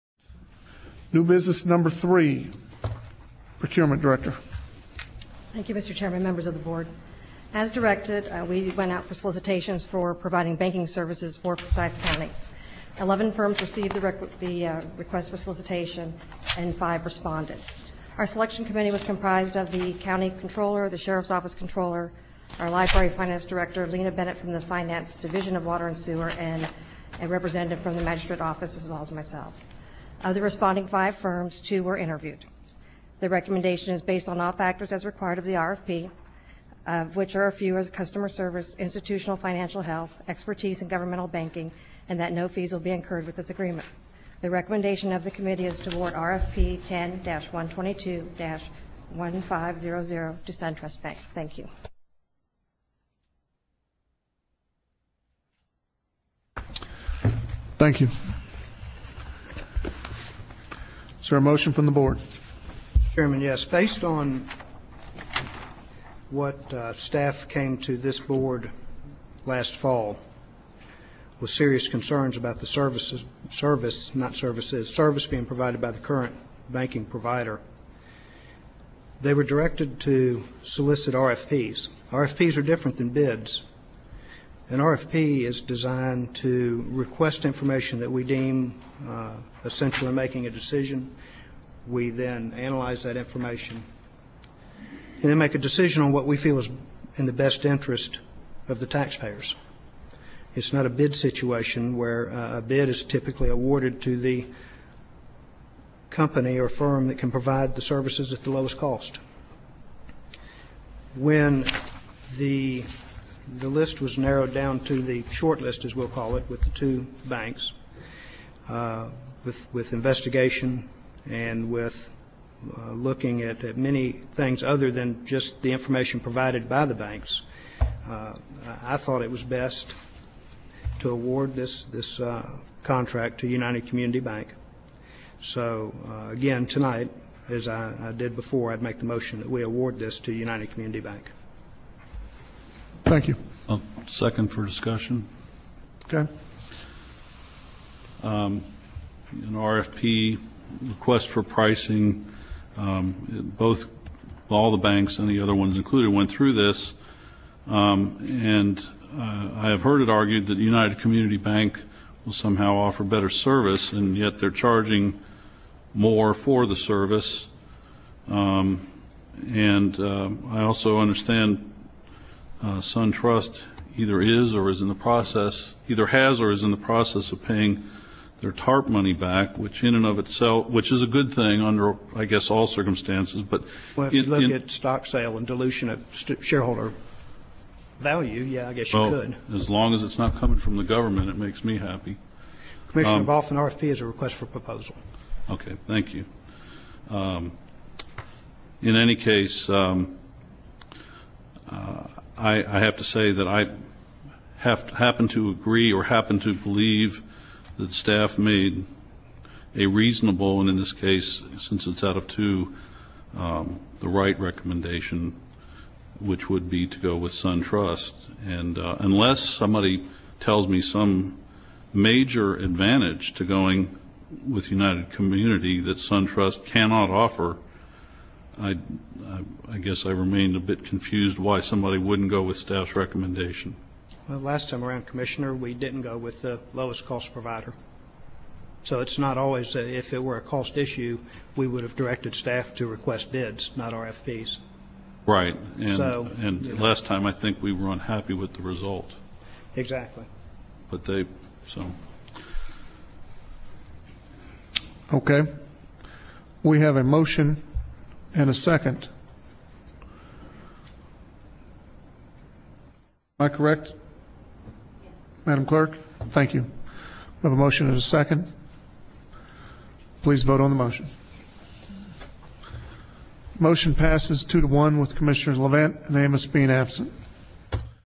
5) Forsyth County Banking 04/07/11
Forsyth County Board of Commissioners meeting April 7, 2011

TAM: New Business #3. Procurement Director.
KUKAROLA: Thank you, Mr. Chairman, members of the board. As directed, we went out for solicitations for providing banking services for Forsyth County. Eleven firms received the request for solicitations and five responded. Our selection committee was comprised of the county comptroller, the sheriff's office comptroller, our library finance director, Lena Bennett from the finance division of Water & Sewer and a representative from the magistrate's office, as well as myself. Of the responding five firms, two were interviewed. The recommendation is based on all factors as are required of the RFP of which are a few: customer service, institutional financial health, expertise in governmental banking, and that no fees will be incurred with this agreement. The recommendation of the committee is to award RFP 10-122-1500 to SunTrust Bank. Thank you.
TAM: Thank you. Is there a motion from the board?
BELL: Mr. Chairman, yes. Based on what, uh, staff came to this board last fall with serious concerns about the services — service, not services — service being provided by the current banking provider, they were directed to solicit RFPs. RFPs are different than bids. An RFP is designed to request information that we deem, uh, essential in making a decision. We, then, analyze that information and then make a decision on what we feel is in the best interest of the taxpayers. It's not a bid situation where a bid is typically awarded to the company or firm that can provide the services at the lowest cost. When the list was narrowed down to the short list, as we'll call it, with the two banks, uh, with investigation and with, uh, looking at many things other than just the information provided by the banks, uh, I thought it was best to award this — this, uh, contract to United Community Bank. So, again, tonight, as I did before, I make the motion that we award this to United Community Bank.
TAM: Thank you.
BOFF: I'll second for discussion.
TAM: Okay.
BOFF: A RFP request for pricing, um, both — all the banks, the other ones included went through this, um, and I have heard it argued that United Community Bank will somehow offer better service and yet they're charging more for the service, um, and I also understand SunTrust either is or is in the process — either has or is in the process of paying their TARP money back which in and of itself — which is a good thing under, I guess, all circumstances, but —
BELL: But, you look at stock sale and dilution of shareholder value — yeah, I guess you could.
BOFF: Well, as long as it's not coming from the government, it makes me happy.
BELL: Commissioner Boff, an RFP is a request for proposal.
BOFF: Okay. Thank you. Um, in any case, I have to say that I happen to agree or happen to believe that staff made a reasonable, and in this case, since it's added, to the right recommendation which would be to go with SunTrust. And, unless somebody tells me some major advantage to going with United Community that SunTrust cannot offer, I guess I remain a bit confused why somebody wouldn't go with staff's recommendation.
BELL: Well, last time around, Commissioner, we didn't go with lowest cost provider. So, it's not always — if it were a cost issue, we would have directed staff to request bids, not RFPs.
BOFF: Right, and —
BELL: So —
BOFF: — last time I think we were unhappy with the result.
BELL: Exactly.
BOFF: But they — so.
TAM: Okay. We have a motion and a second. Am I correct, Madam Clerk?
BUSH: Yes.
TAM: Thank you. We have a motion and a second. Please vote on the motion. Motion passes two to one with Commissioners Levent and Amos being absent.